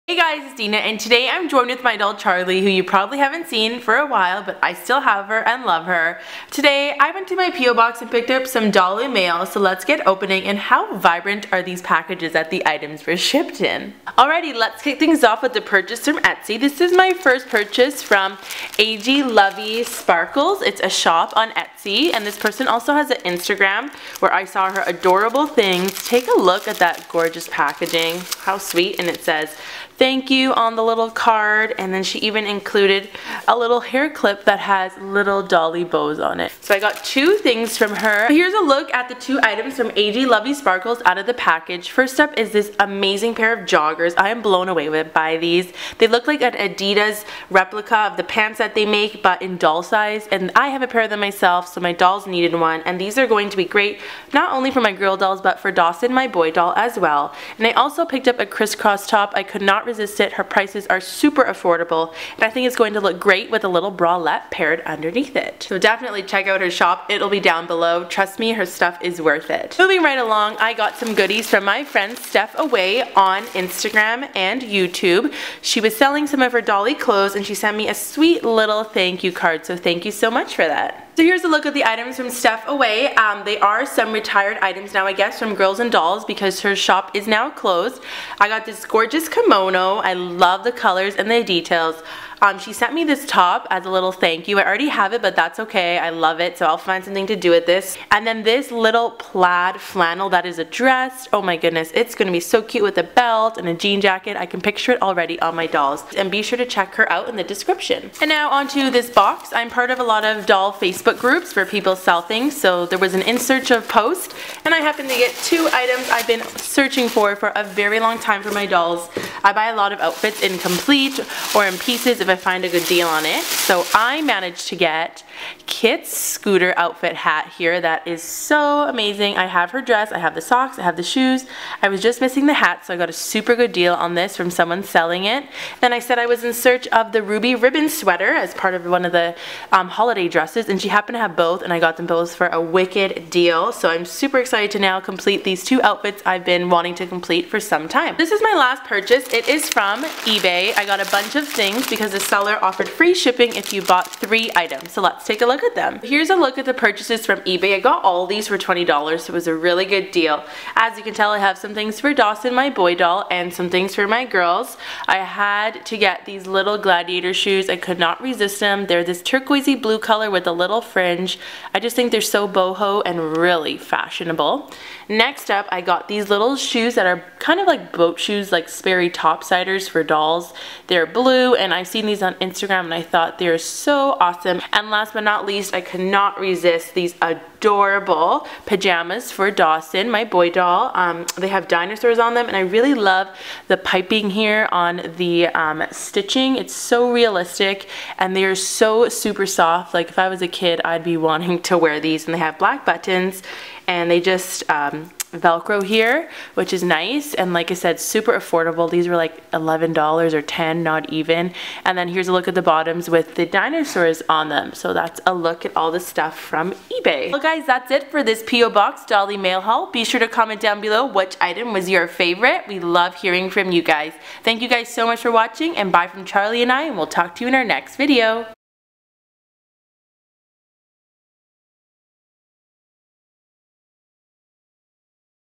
Opening HUGE American Girl Doll Mail HAUL | Etsy & Ebay
I clearly have an obsession with my buying my AG dolls cute and stylish clothes! Gosh my dolls dress better than me! Hope you enjoy and let me know which item was your favourite :)
Etsy shops featured in this video: